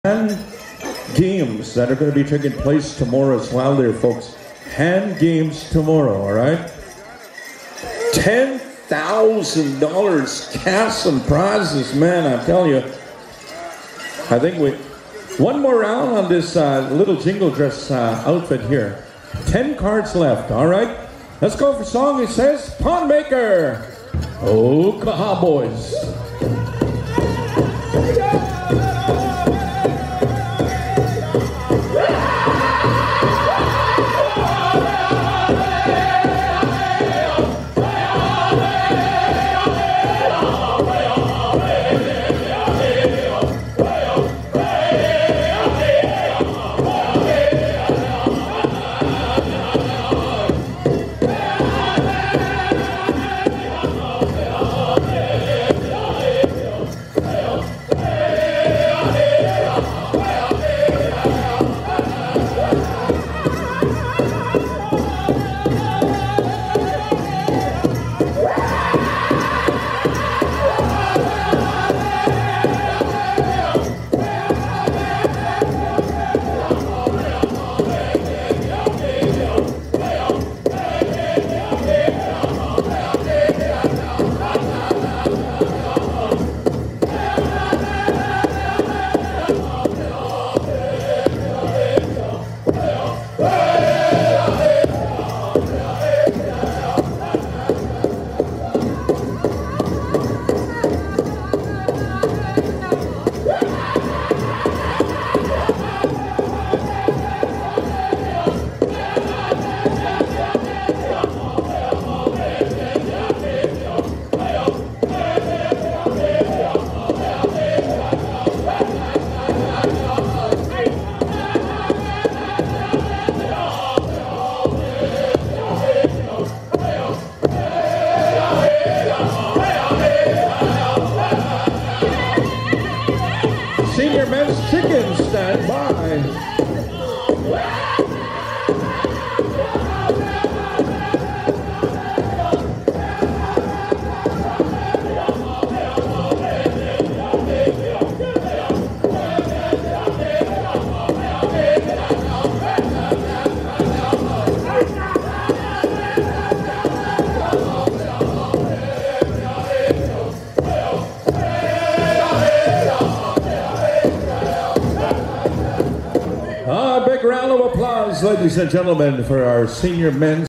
Ochapowace Powwow 2019 Sr. Men's Grass Friday
Senior Adult Men's Grass Dance... Friday Evening... Poundmaker Singers...